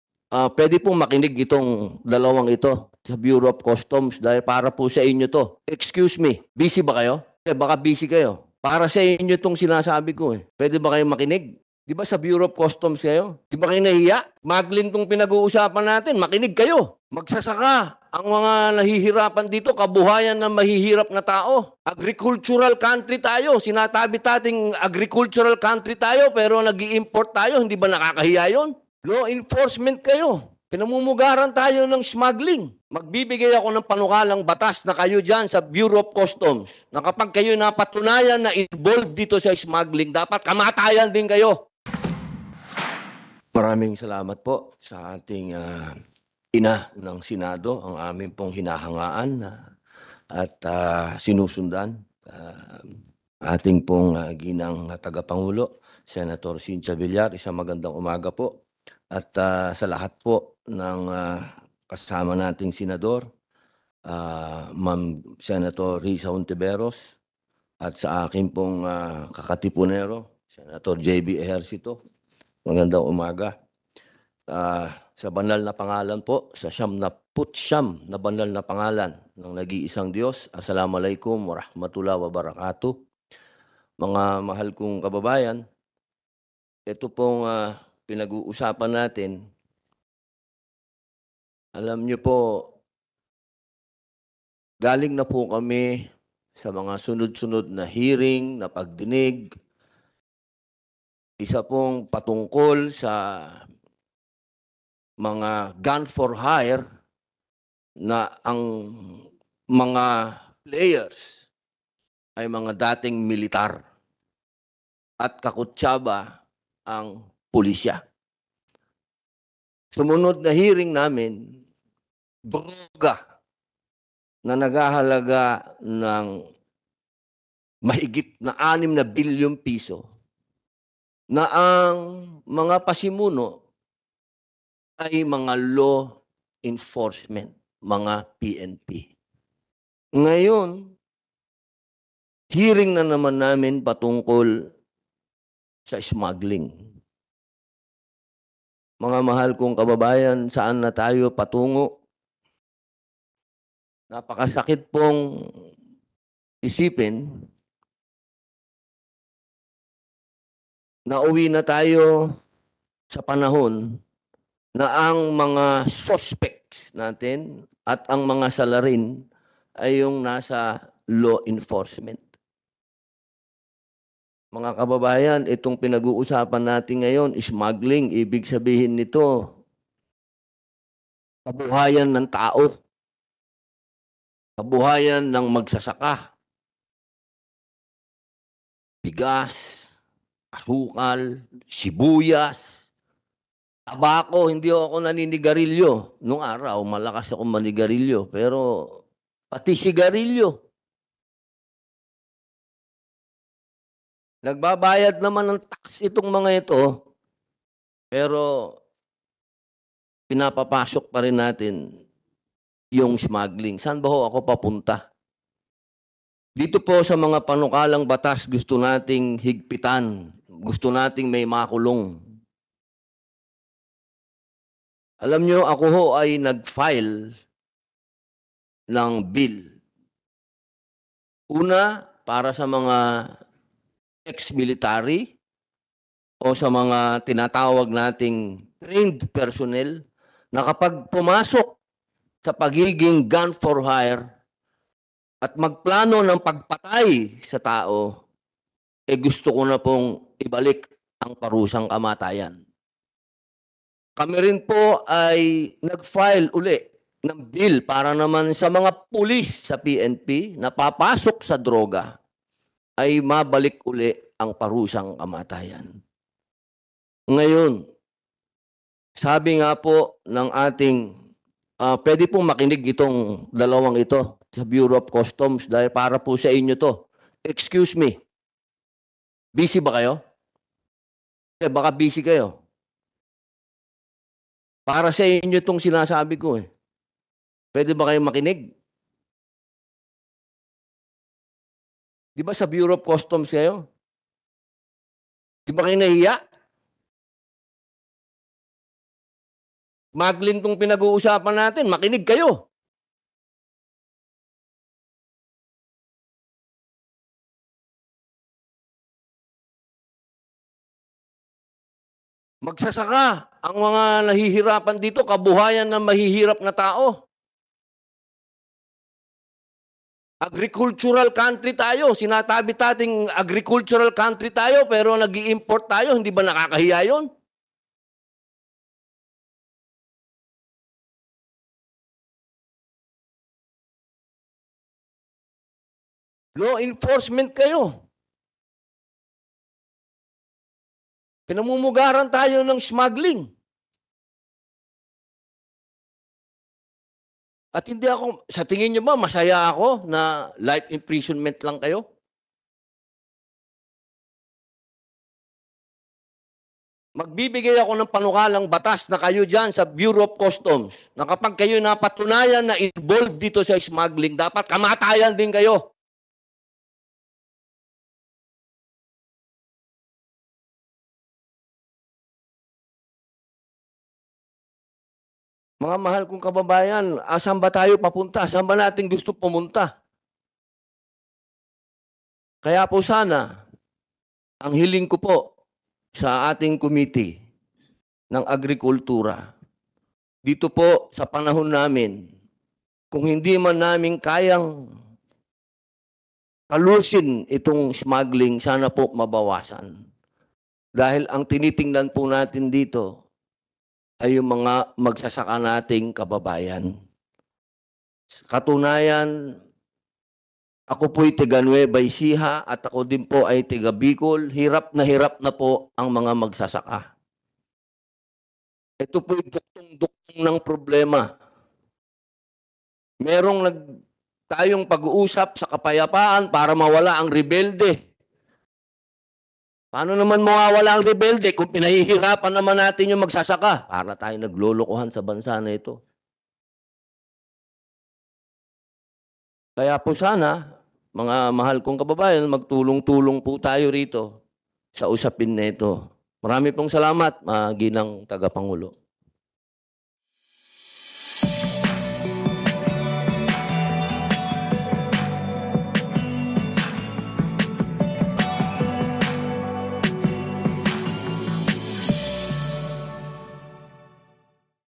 Sen. Robin Padilla, gagawa ng batas na kamatayan, ang parusa sa customs na sangkot sa smuggling
Sen. Robin Padilla, gagawa ng batas na kamatayan, ang parusa sa beauru of customs na sangkot sa  ! L L E G A L smuggling